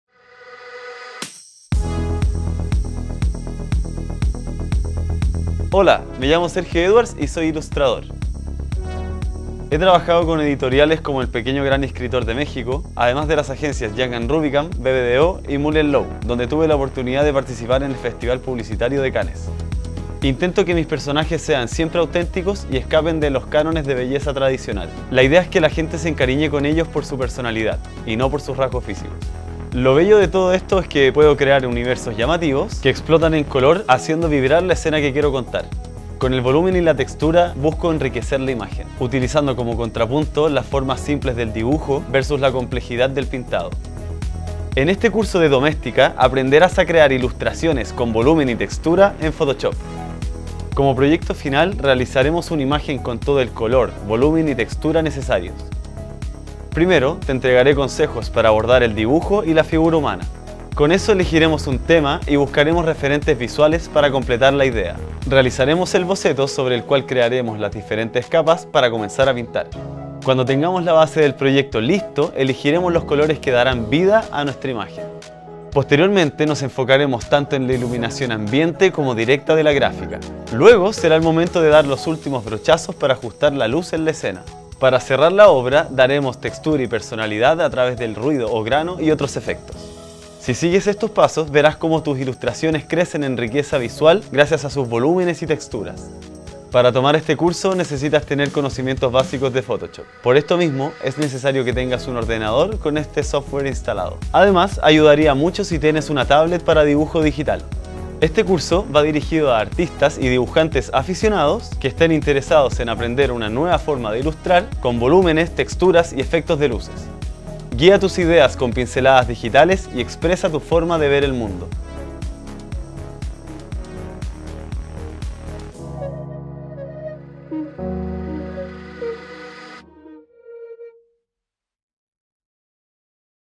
Ilustración Digital con Volumen y Textura | Un curso de Sergio Edwards
Aprende a darles una apariencia 3D a tus ilustraciones usando técnicas de luces, sombras y color en Photoshop

Para ilustrar un gran personaje no basta con recrear sus facciones. Con tu imaginación y un buen manejo técnico también puedes definir su personalidad y sus gestos para que expresen lo que tienes en mente de una manera tangible y evidente.

En este curso, el ilustrador Sergio Edwards te enseñará a crear una ilustración digital en su particular estilo: dándole una apariencia 3D con luces y sombras, y con personajes expresivos y coloridos. Para ello, te mostrará todos los pasos técnicos y te compartirá sus pinceles para que logres plasmar todo lo que tienes en mente.